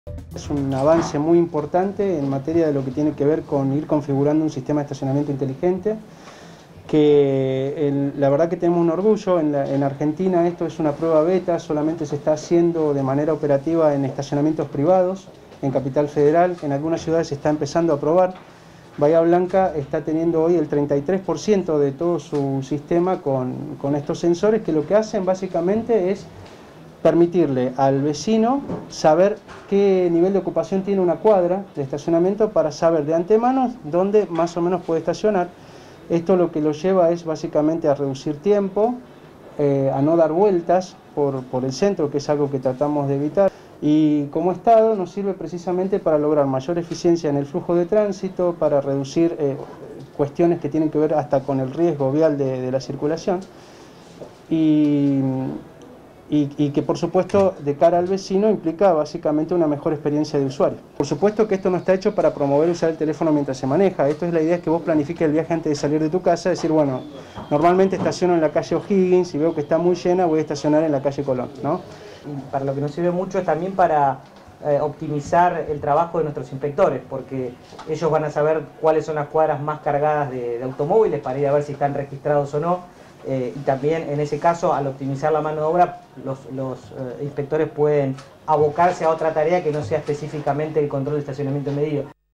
Se instalaron más de 2300 sensores en boxes de estacionamiento
Los mismos fueron ubicados en forma estratégica en 164 manos para maximizar la eficiencia de conexión del propio sensor.